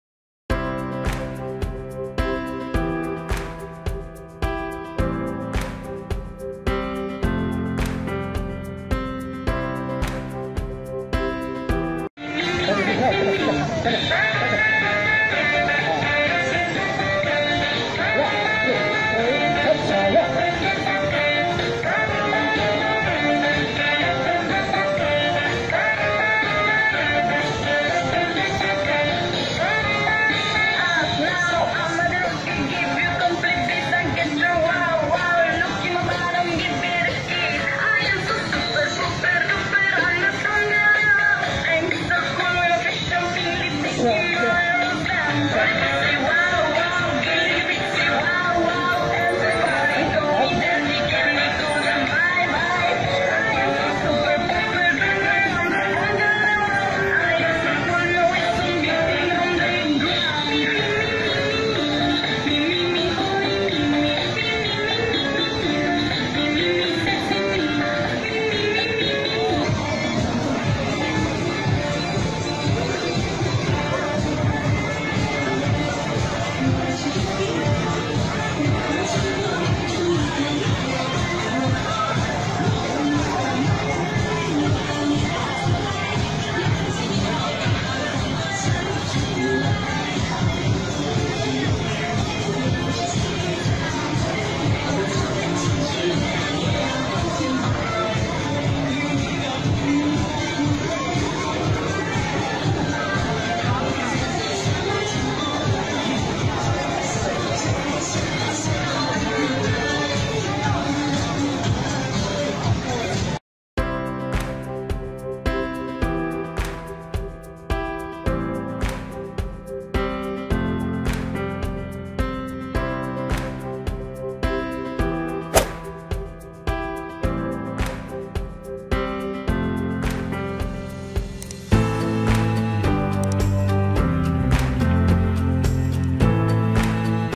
China, Shenzhen بعد الشغل
من مجموعة فيديوهات كومك حول العالم  China, Shenzhen المتعه انك تشوف الناس ديه عايشه حياتها بعد الشغل وينسوا هموم الشغلم